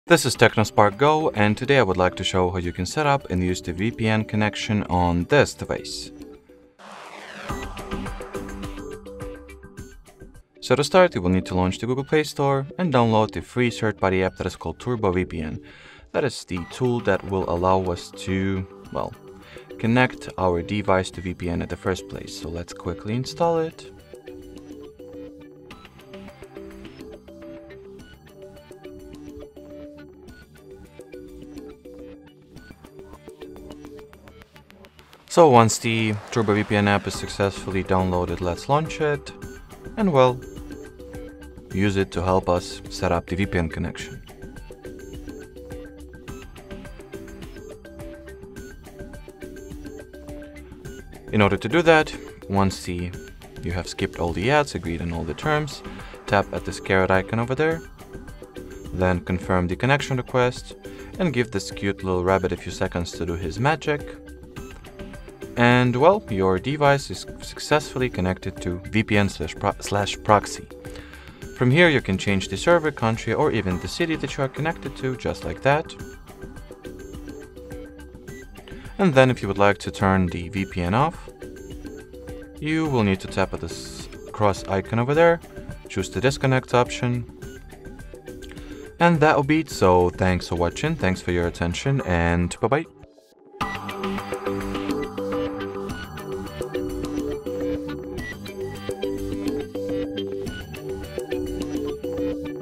How To Set Up VPN on Tecno Spark Go | Secure Internet Connection 🔒
Protect your online privacy and secure your internet connection with a Virtual Private Network (VPN) on your Tecno Spark Go! In this comprehensive guide, we'll walk you through the step-by-step process of setting up a VPN on your device.

How to set up VPN on Tecno Spark Go